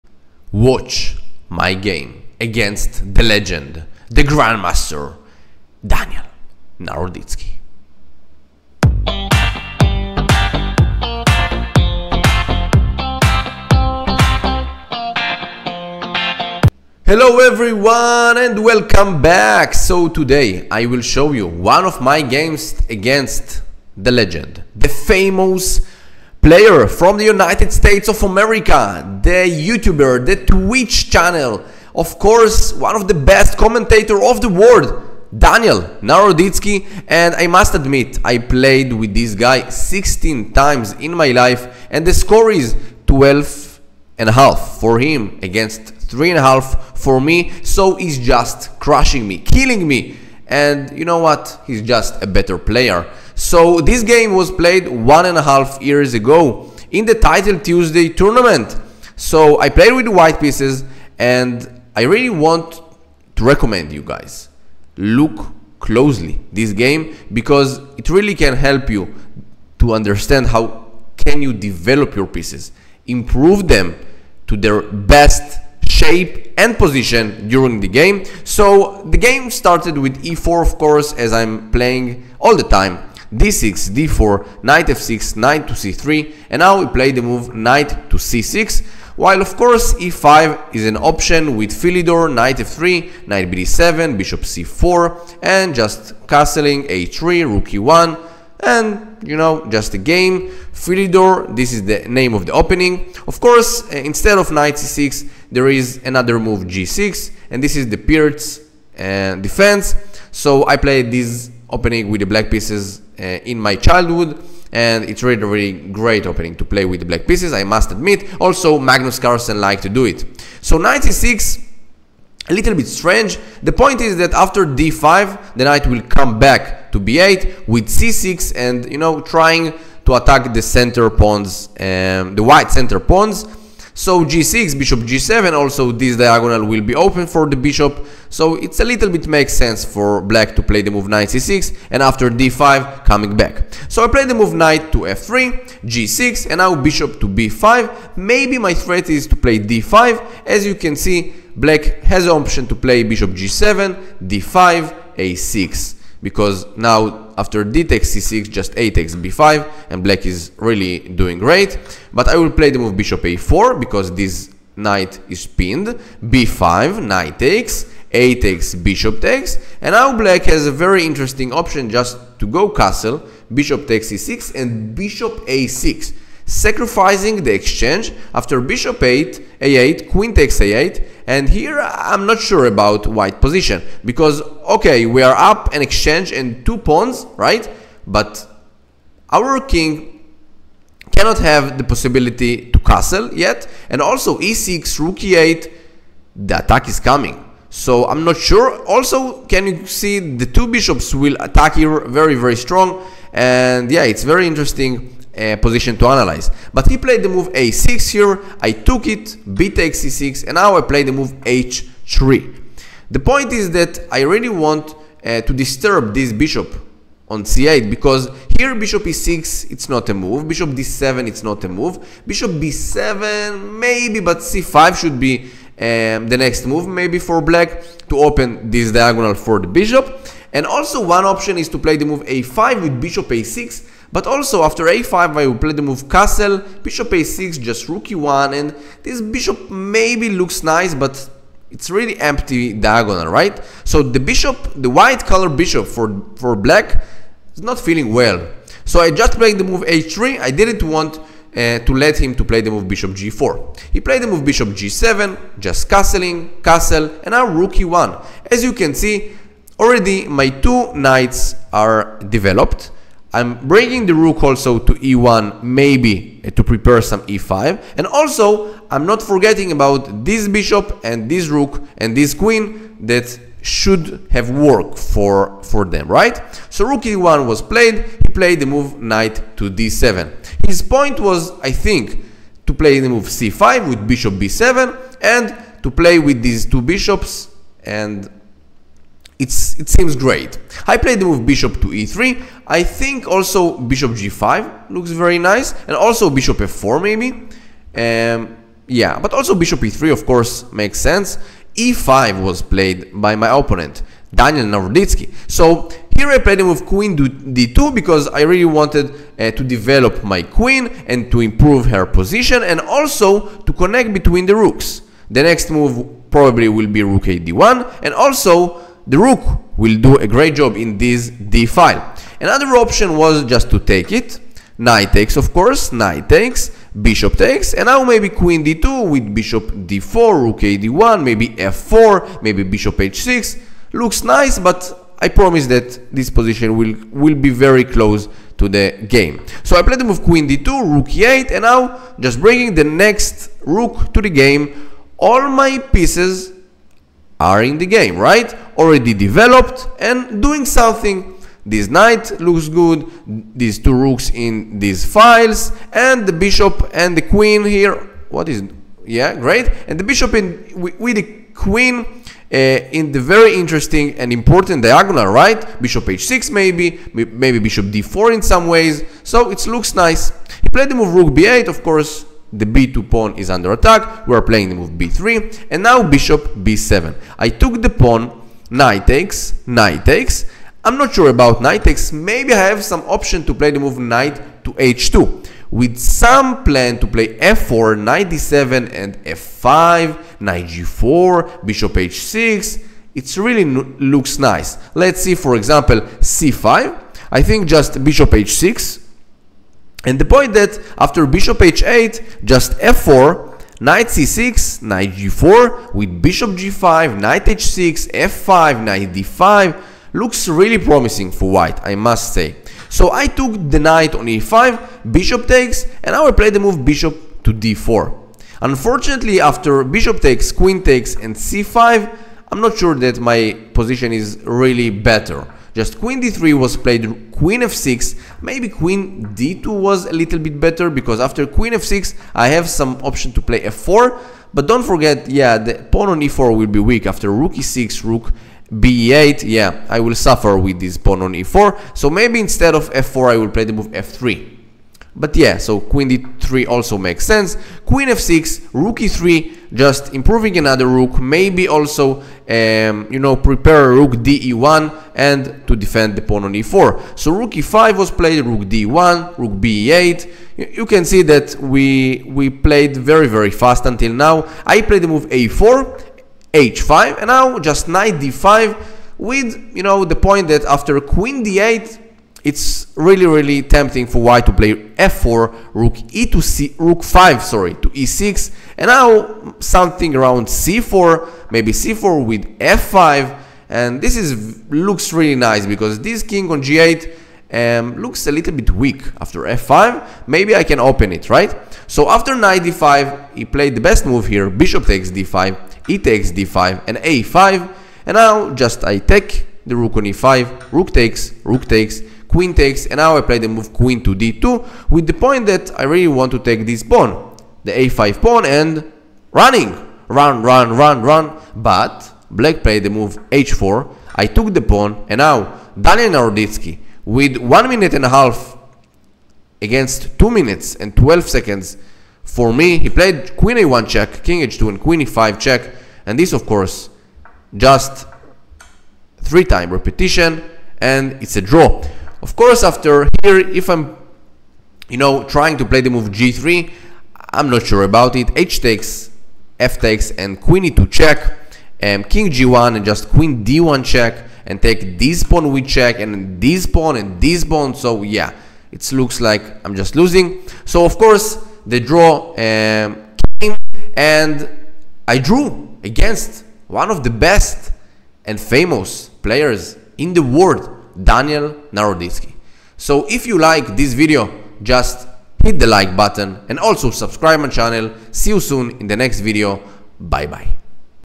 Can I Survive Against GM Daniel Naroditsky?
In this thrilling chess match, I face off against the incredible GM Daniel Naroditsky. Can I withstand the pressure and hold my own against one of the best? Watch the intense moves and see how this battle unfolds!

🔹Enjoy my videos?